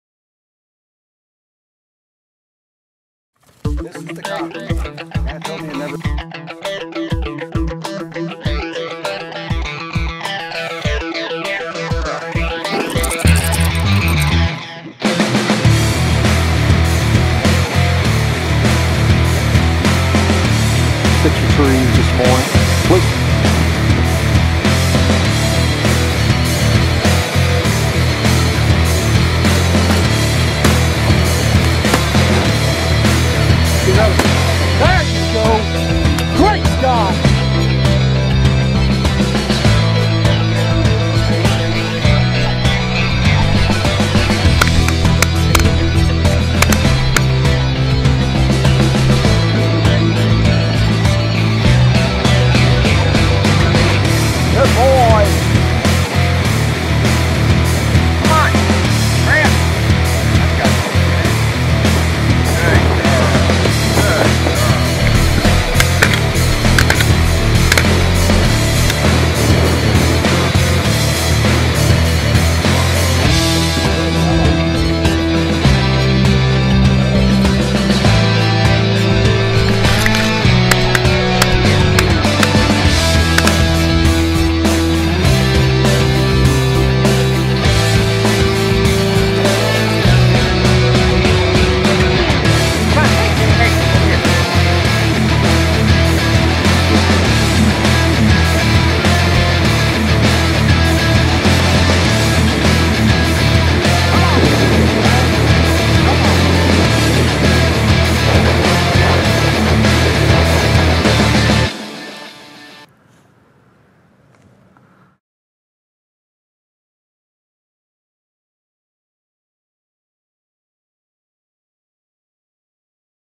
Just Retrievers
The Plum Ridge Social Club dogs do their thing.